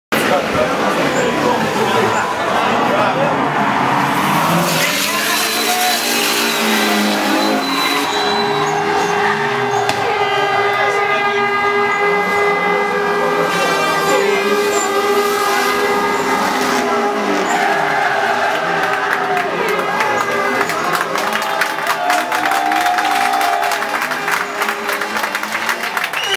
Ela Koko vs Doros 3rd Drift 2011
BMW E30 325i Turbo
Twin Drift
Best 4
Full Built and Tuned by C.C. Racing
Limassol, Cyprus